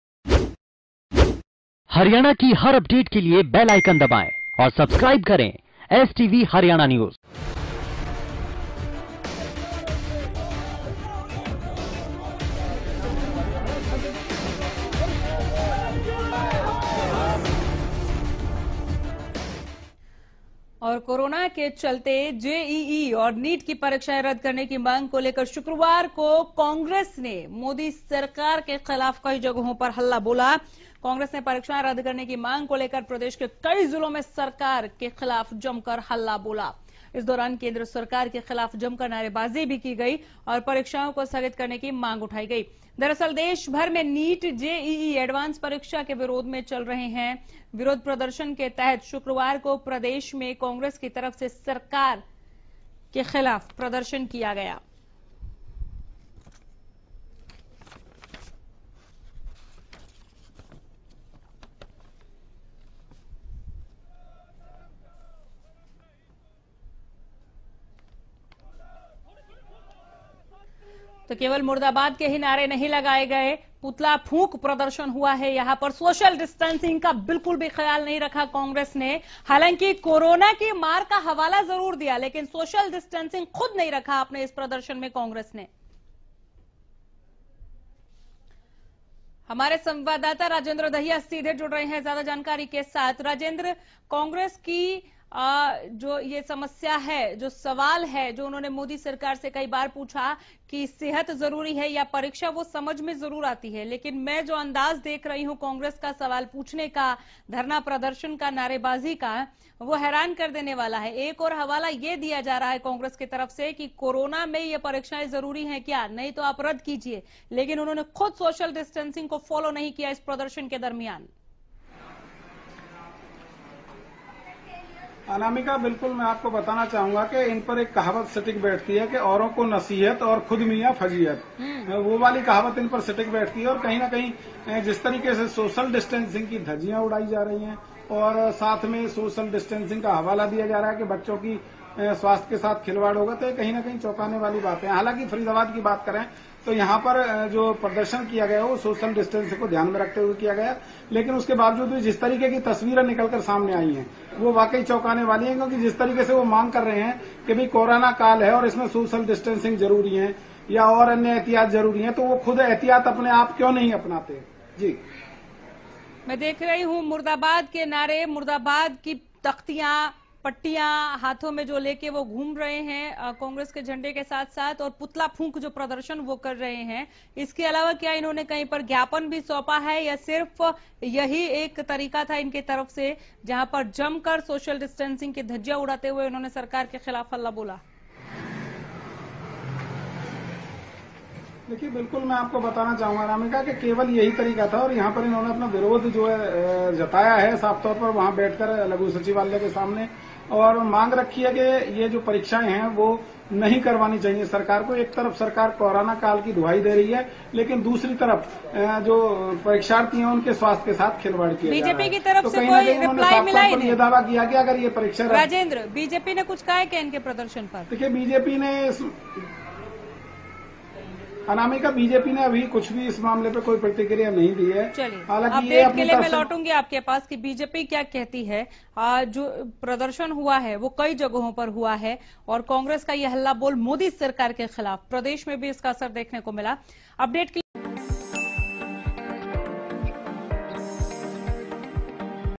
NEET-JEE परीक्षा के विरोध में सड़कों पर उतरी युवा कांग्रेस...परीक्षा जरूरी या सेहत?
कोरोना संक्रमण के चलते नीट और जेईई परीक्षाएं रद्द करने की मांग को लेकर शुक्रवार को कांग्रेस ने केंद्र की मोदी सरकार के खिलाफ हल्ला बोल किया.